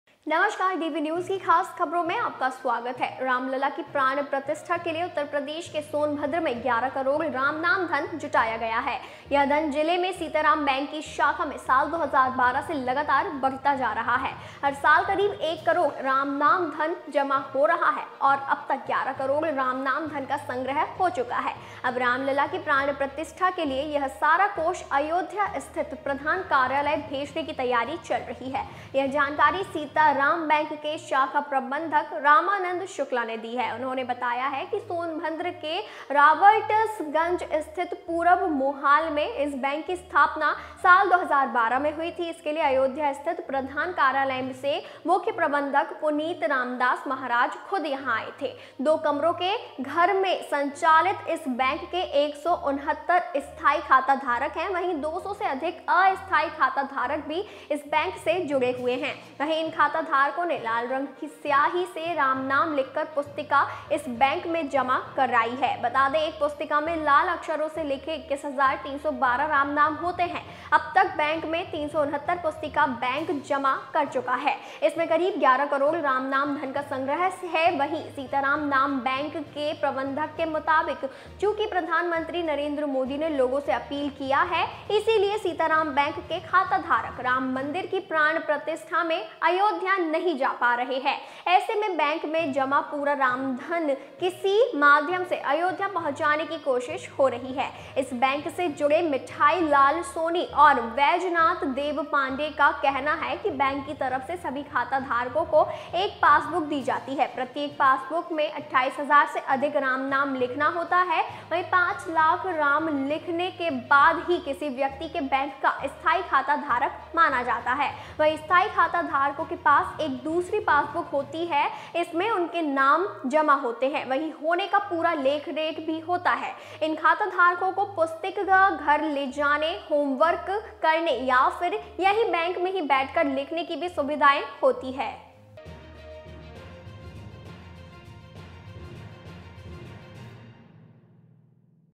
Ram Mandir: रामलला की प्राण प्रतिष्ठा के लिए जुटाया गया 11 करोड़ 'रामनाम धन', इस बैंक में जमा होगा
Rs 11 crore Ramnam money has been collected in Sitaram Bank of Sonbhadra. In the last 11 years, more than 400 account holders of this bank have deposited this invaluable fund in the bank. Now at the time of Ram Lalla's consecration, this entire collection will be deposited in the main office in Ayodhya. Preparations for this have been completed.